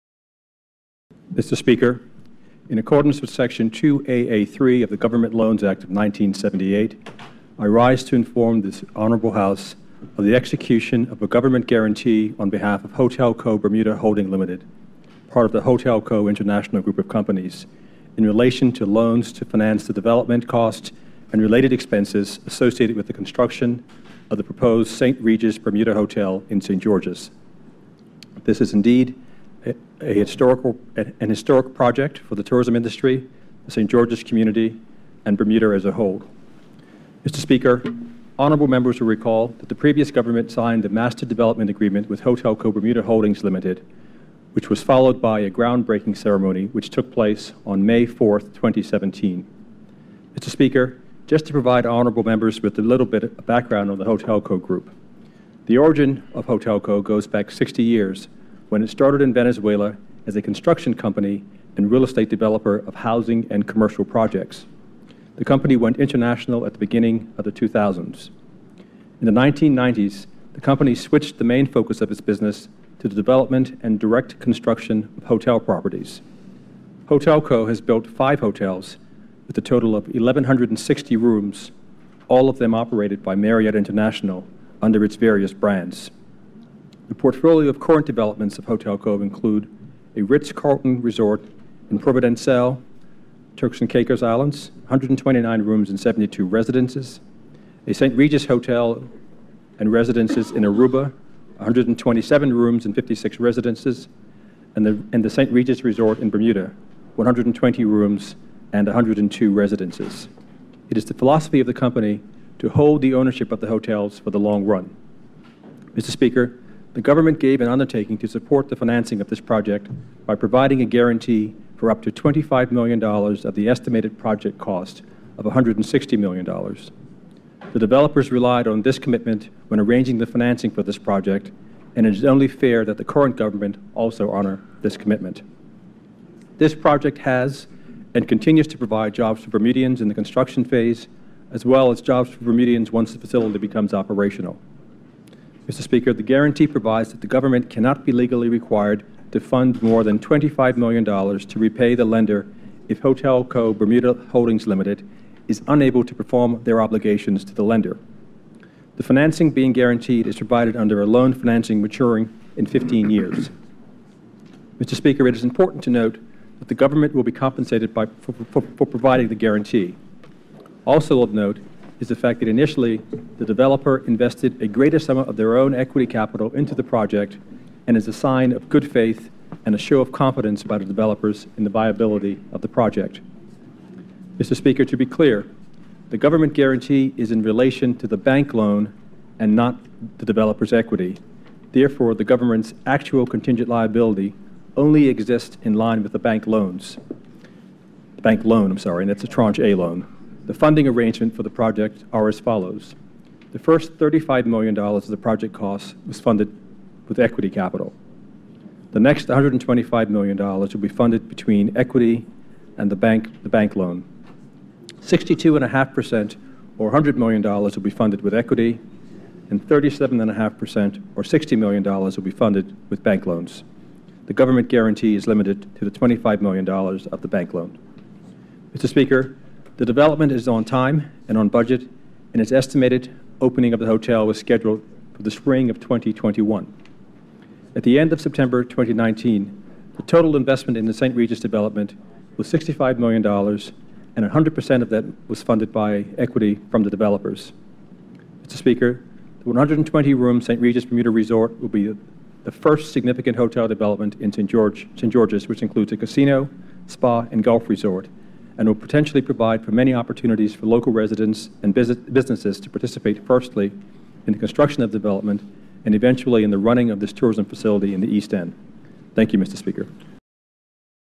Government Guarantee Hotelco Bermuda Holding Ltd
The Hon. Curtis Dickinson JP, MP, Minister of Finance delivers a Ministerial Statement to the House of Assembly on the Government Guarantee Hotelco Bermuda Holding Ltd on Friday, November 1, 2019.